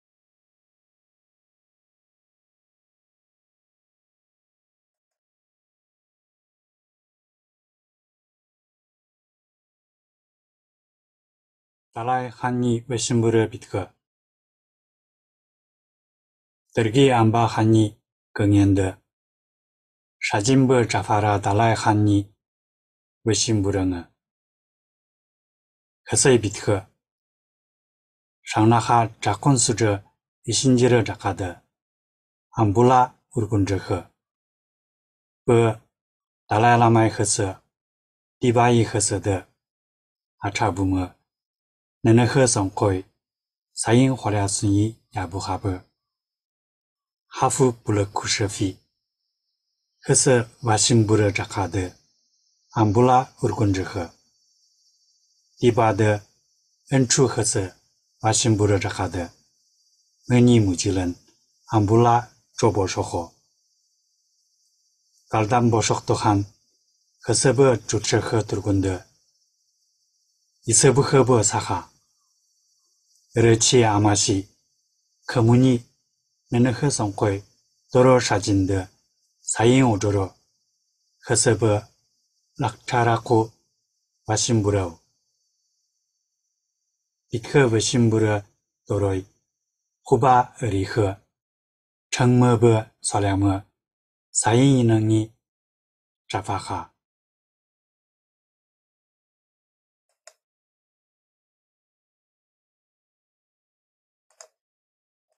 マンジュ語奏摺朗読見本2-達賴汗-奏請頒旨並進獻琥珀數珠氆氌-康熙36年02月24日(1697-03-16)滿文
マンジュ語朗読見本2-達賴汗-奏請頒旨並進獻琥珀數珠氆氌-康熙36年02月24日(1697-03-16)（※未經許可，不得擅自轉載 。 僅限於本學期內公開。）
【漢譯文】
〔康熙三十六年二月二十四日〕【※原滿文摺漢文貼簽字】
達賴汗奏章
〔奏章譯〕【※原滿文摺漢文貼簽字】
至尊大汗（dergi amba han或譯作東方大汗）睿鑒，持教達賴汗奏。收到上諭及賞賚緞八匹，不勝歡喜。我等遵奉達賴喇嘛之旨，第巴之旨，如前協和而行，蒙睿鑒洞照，頒降諭旨，不勝歡欣。別降諭旨于第巴，我等之心，大為困苦。噶爾丹博碩克圖汗以違悖諭旨之故，得知被懲處。自今以後,伏望仍如前于道法協和之恩綸，頒示無絕。以奏書禮，吉日並獻琥珀素珠,氆氌。